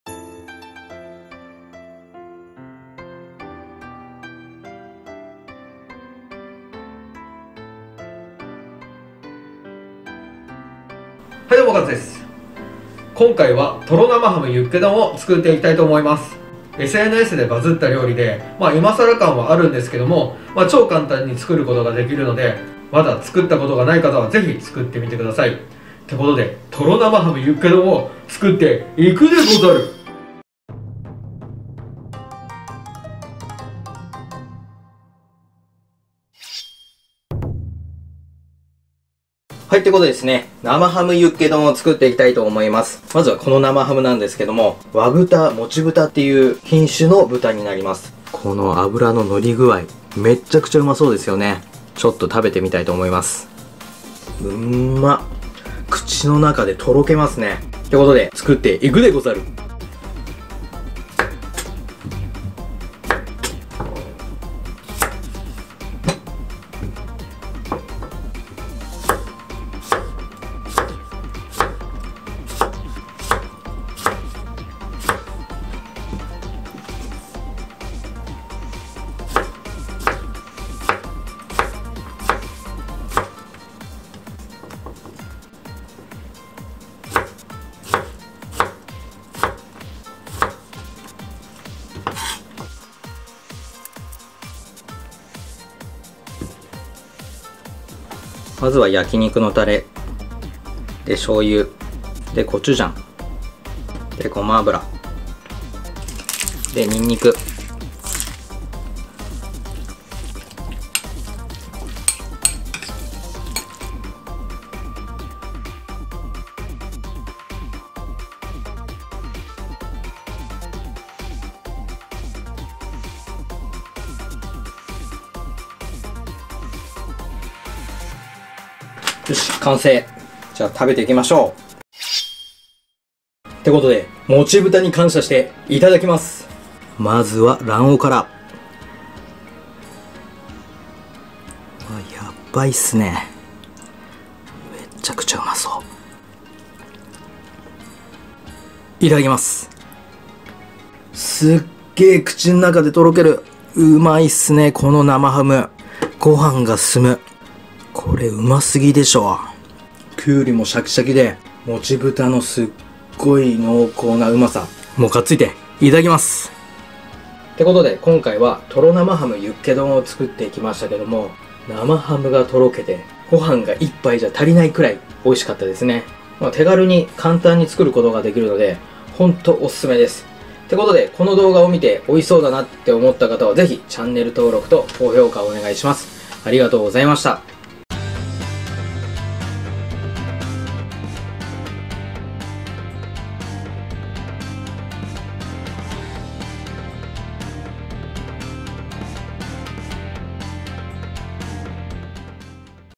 【1分で作れます】和豚もちぶたの生ハムユッケ丼の作り方
今回はＳＮＳで話題になった「生ハムユッケ丼」を作ってみました。
手軽で簡単に作ることが出来るのでおすすめです。

▼目次
0:00　オープニング
0:40　食材紹介
1:00　調理開始
1:54　盛り付け
2:12　実食

▼材料
　・生ハム　：薄切り8枚
　・キュウリ：半分
　※動画で使用した生ハムは、「和豚もちぶた」というブランド豚です。

▼漬けダレ
　・焼き肉のタレ：小さじ1
　・醤油：小さじ1
　・ごま油：小さじ1
　・コチュジャン：5cm
　・ニンニク：2cm